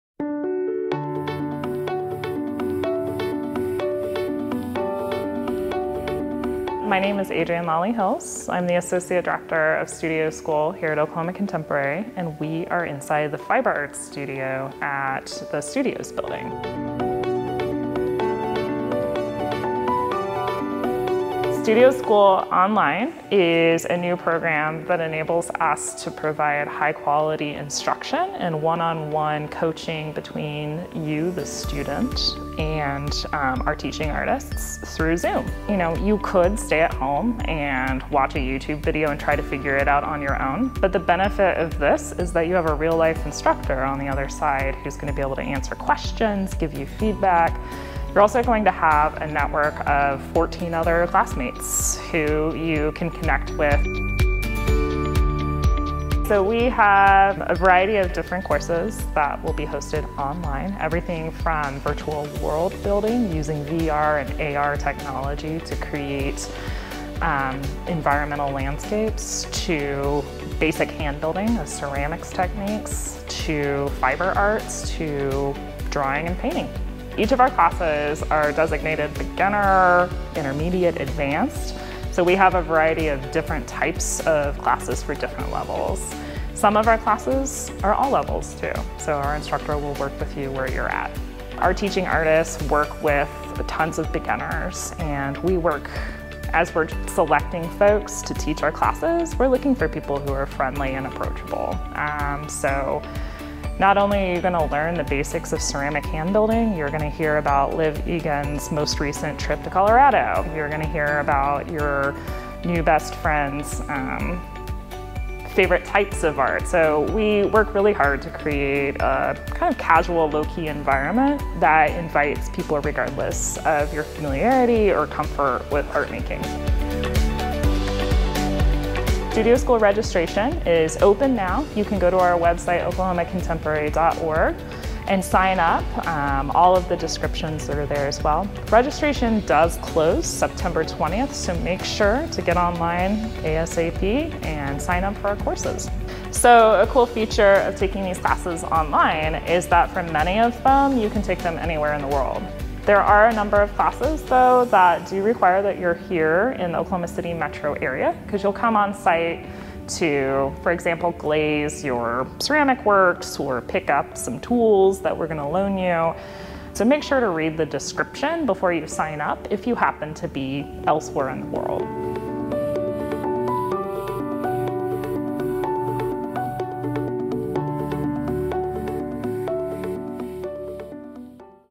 Studio School Online
Learn more about Oklahoma Contemporary's new virtual classes and workshops.

If you've been looking for the opportunity to expand your creative horizons during this time of physical distancing, Oklahoma Contemporary's virtual 10-week classes and workshops will help you do just that. Taught by experienced working artists, course offerings range from traditional media like drawing, painting and collage to digital animation, virtual worldbuilding and a whole lot more. It's time to save your spot.